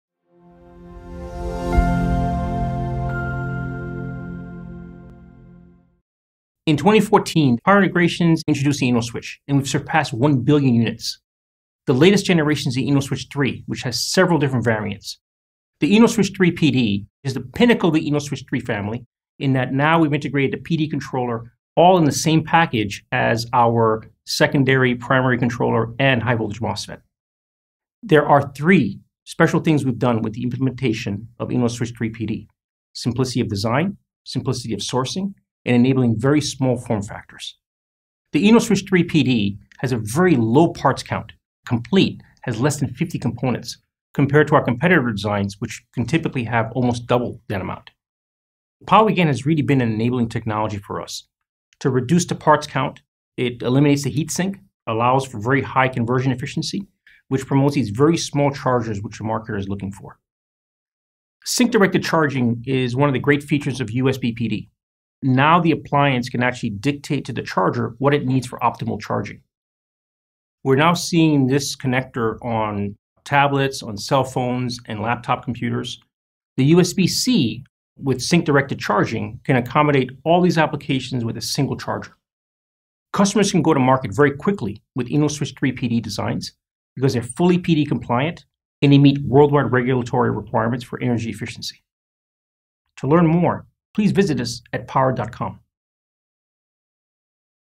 Introduction to InnoSwitch3-PD
InnoSwitch3-PD is our first flyback switcher IC with USB PD controller built in. This fully integrated solution combines USB PD, PPS, PowiGaN and FluxLink technologies to maximize efficiency and slash component count in adapters and chargers. Director of New Product Definition Roland Saint Pierre explains.